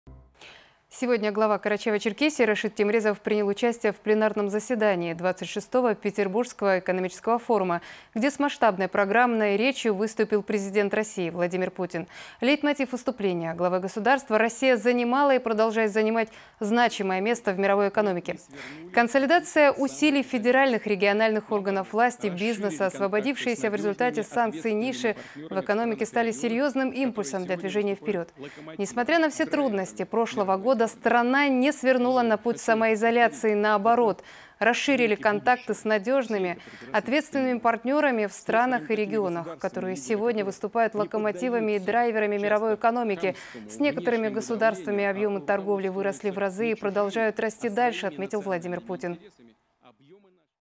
Пленарное заседание экономического форума в Санкт-Петербурге прошло при участии главы КЧР
Близится к завершению 26-й Петербургский Международный экономический форум, участие в котором принимает и глава КЧР Рашид Темрезов. Сегодня, 16 июня, на пленарном заседании выступил Президент России Владимир Путин. Он отметил, что наше государство продолжает расширять сотрудничество с надёжными и ответственными партнёрами, чьи страны и регионы играют далеко не последнюю роль в мировой экономике. По словам Владимира Путина, значительно увеличились объёмы торговых операций с рядом государств, лидеры которых руководствуются собственными здравыми национальными интересами.